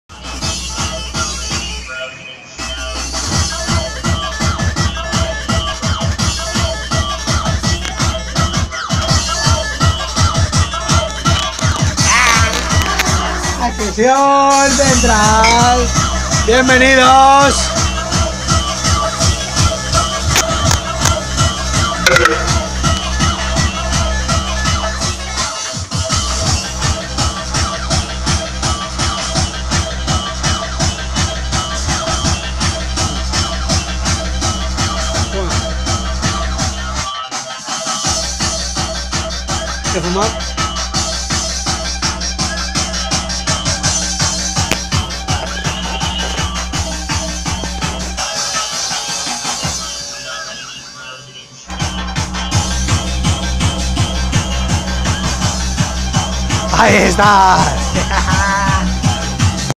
Dj Paco Cuenca A.k.A Panocho
Pinchando mano a mano con mi amigo y futuro Dj Fernando Matamoros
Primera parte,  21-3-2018